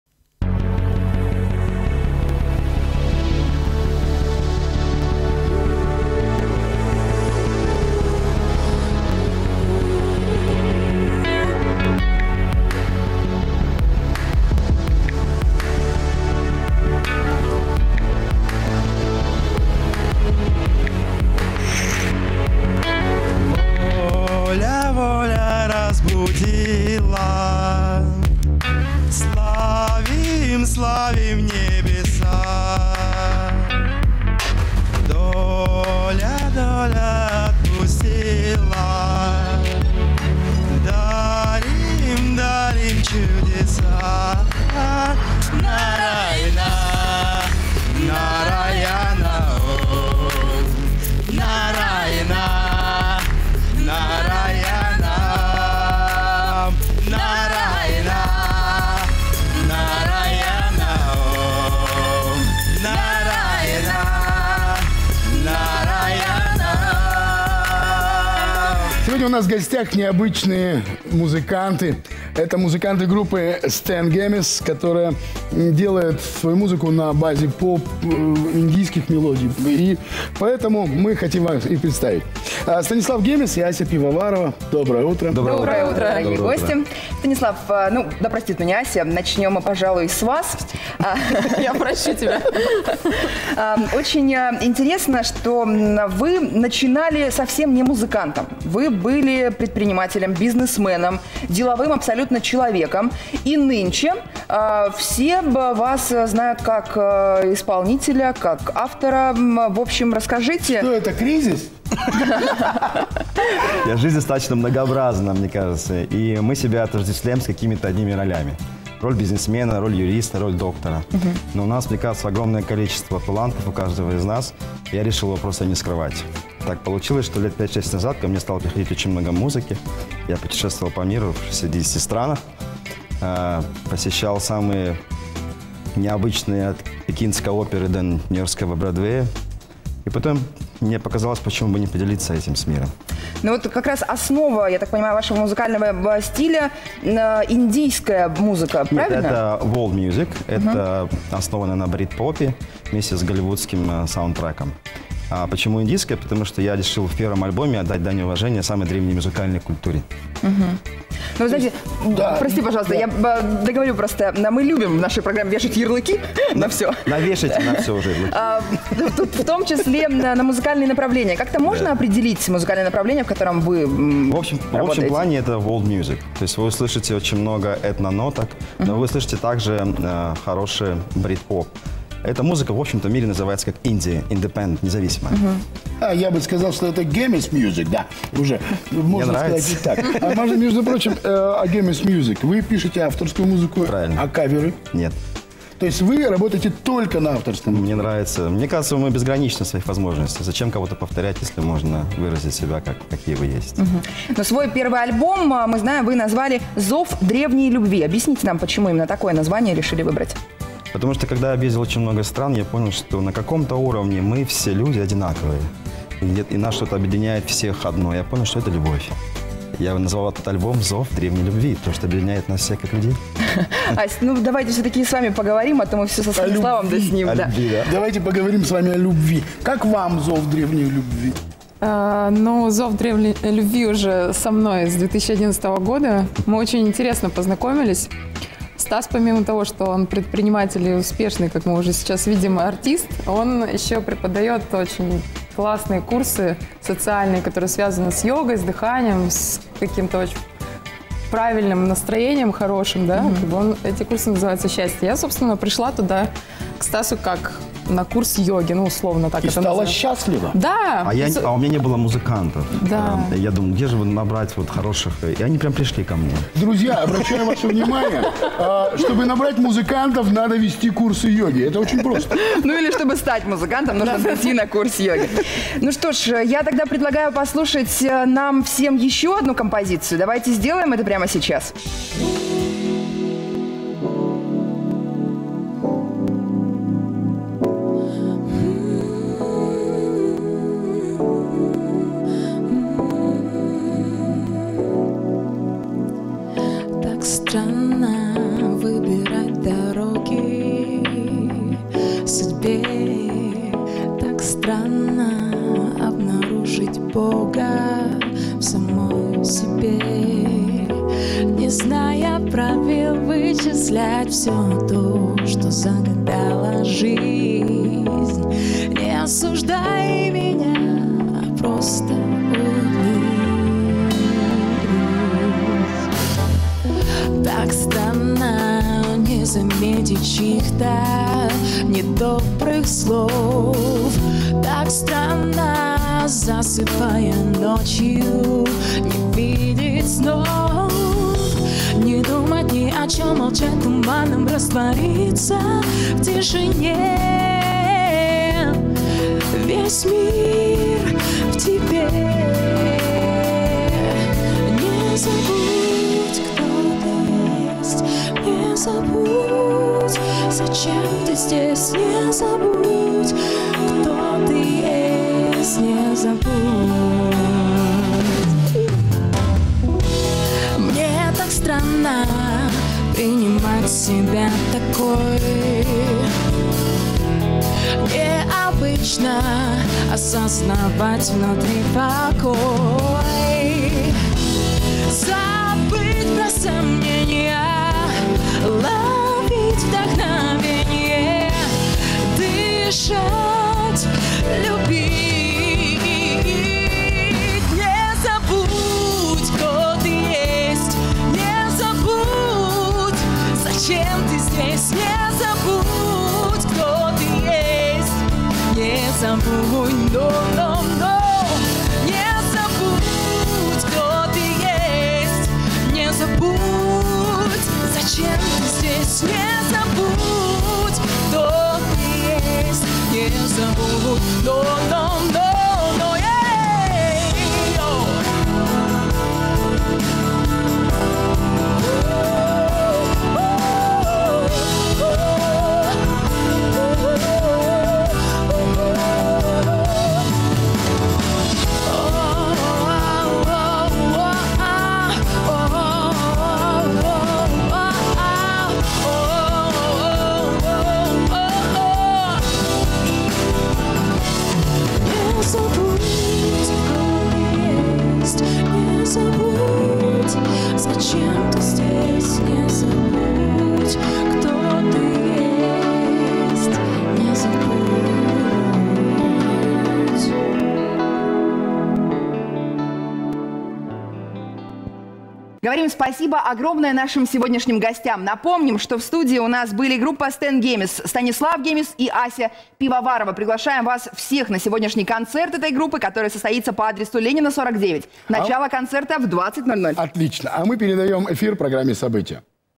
Хорошее музыкальное настроение от этно-группы Stan Gemes (04.03.15)
Сегодня у нас в гостях побывали очень необычные гости — музыканты группы Stan Gemes Станислав Гемес и Ася Пивоварова. Они делают поп-музыку и электронику на базе индийских мелодий. Знакомьтесь и слушайте!